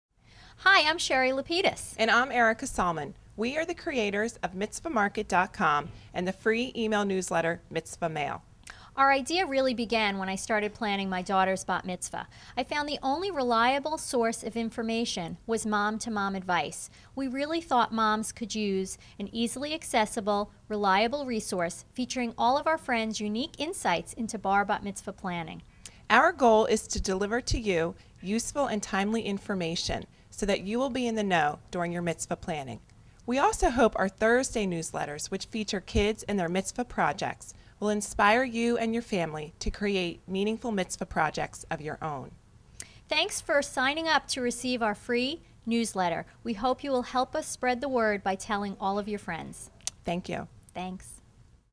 Welcome to MitzvahMarket.com
Welcome video for MitzvahMarket.com featuring co-founders Sheri and Erica.